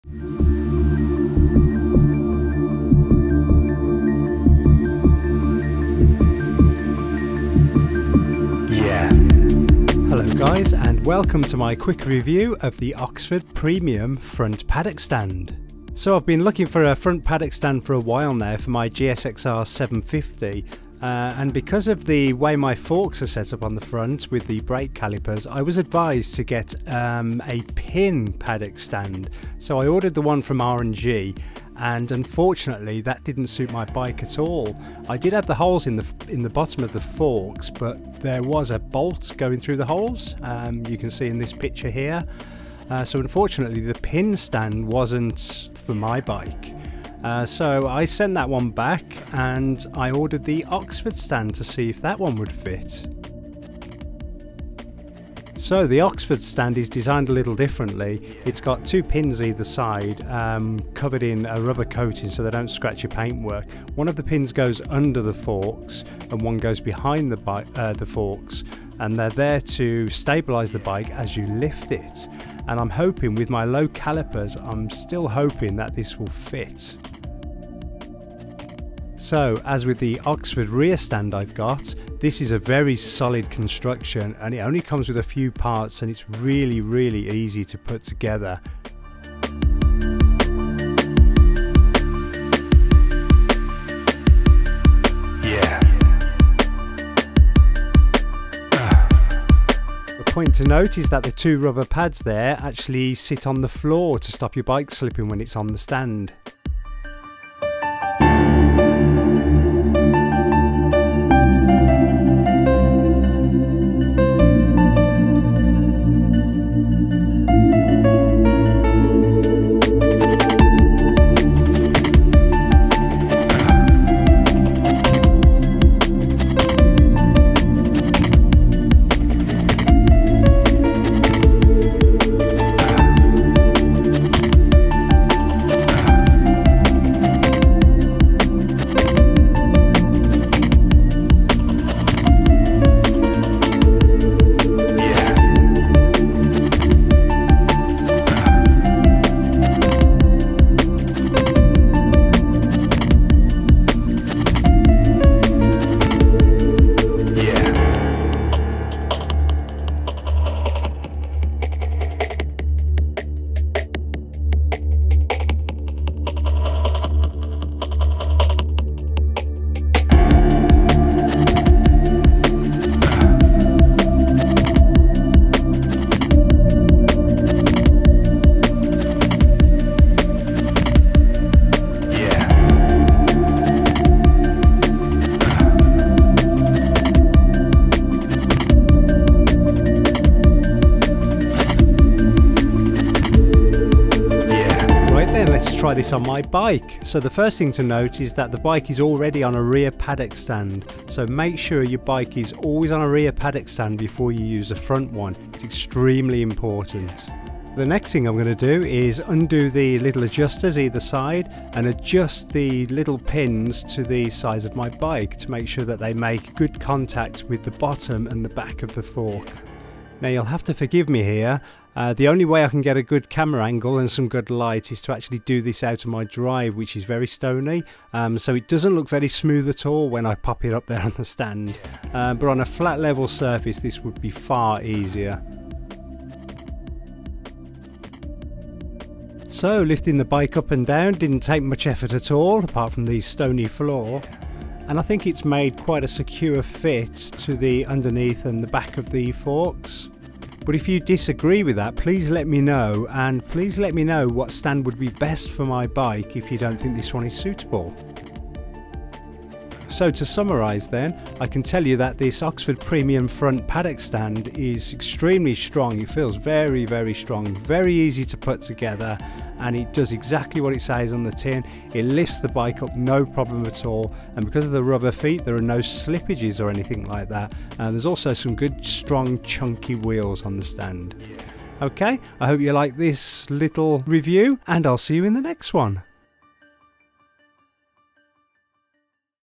Oxford Premium Front Paddock Stand - Full Test and Review
Hi Guys, I've just picked up the Oxford Premium Front Paddock Stand for my 2016 Suzuki GSX-R750 and thought I'd share a little review.

I have had trouble finding a good stand for my Motorbike and have tried he R&G Pin Stand which was not suitable.  I'm hoping this one is better and I'd be interested to read your thoughts.